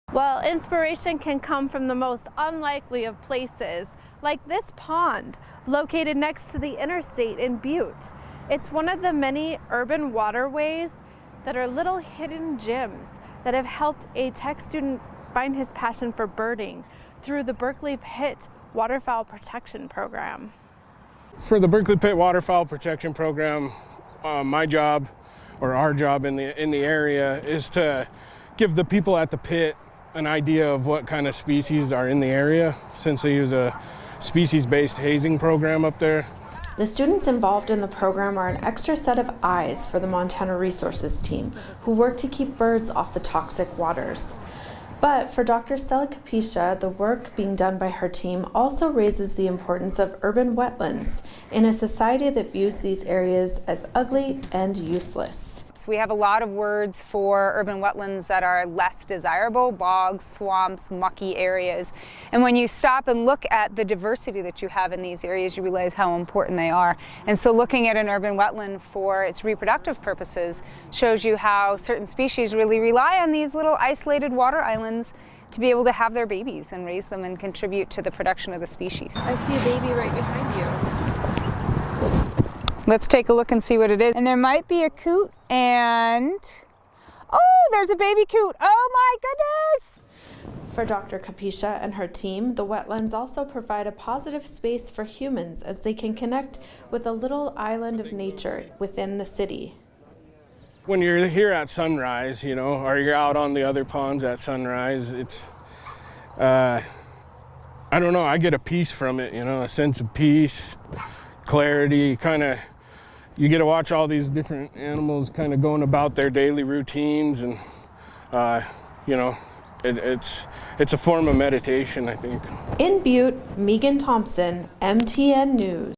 Butte birder helps keep migratory birds off toxic Berkeley Pit
Shawn Moorman trains Montana Tech students to help Montana Resources identify species of migratory birds that might land on the toxic waters of the Berkeley Pit.